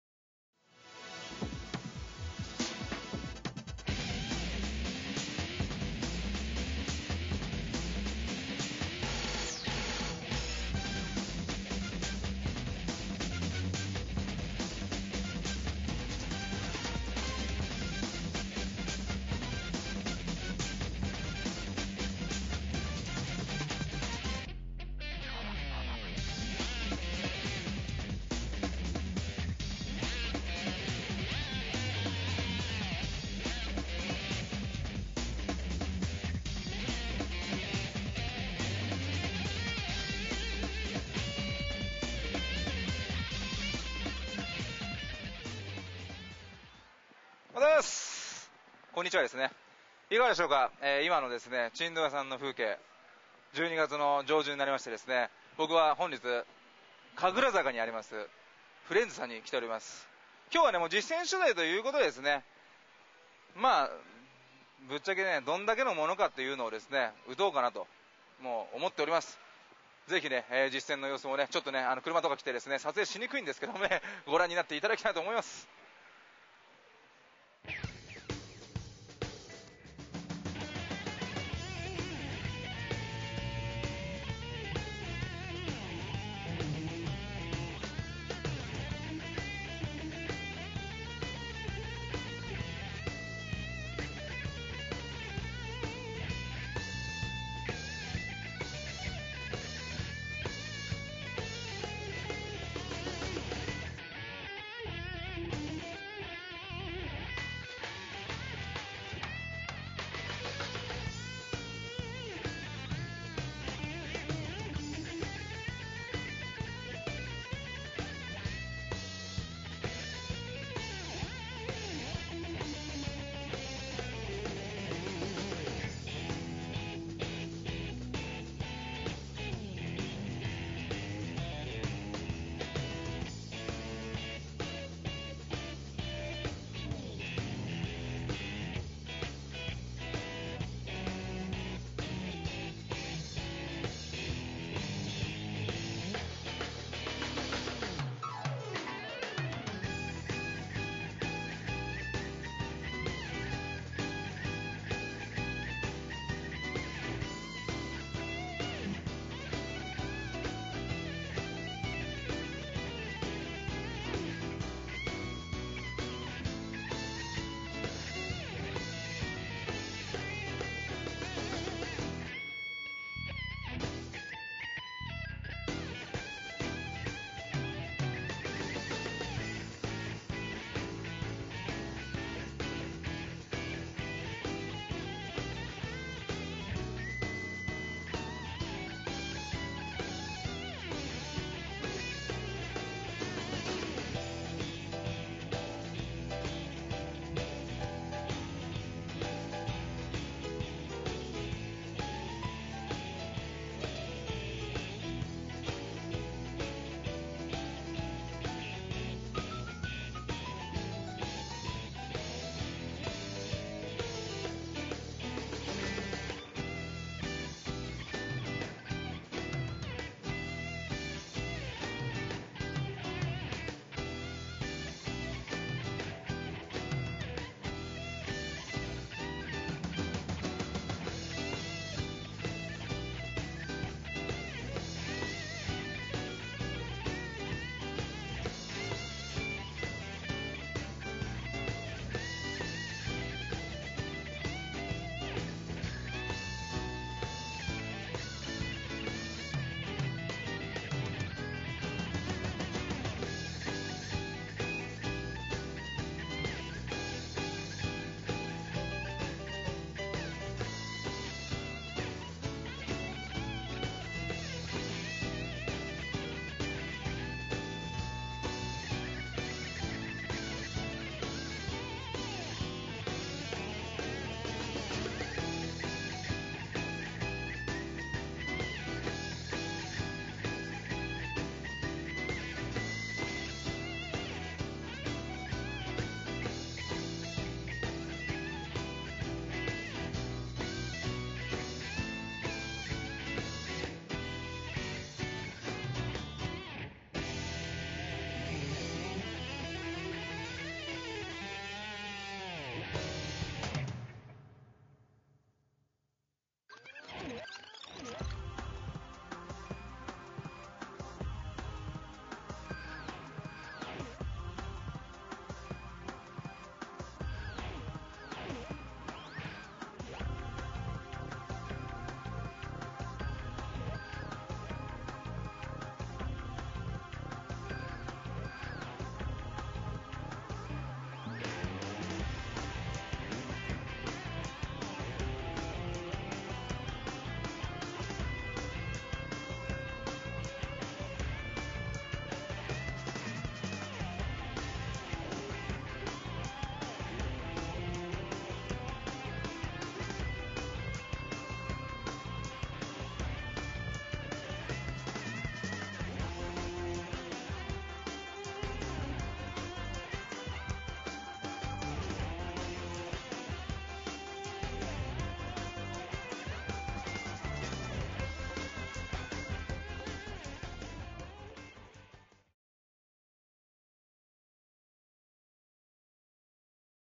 【P-martTV】景品獲得バラエティDashman フレンズ 2011年12月10日 初開催
東京都新宿区神楽坂にある「フレンズ」にて
P-martTV のDASHMAN初取材！！
P-martTVメンバーよーへいが店内を盛り上げました。

パチンコ・パチスロ パソコンテレビ
「P-martTV」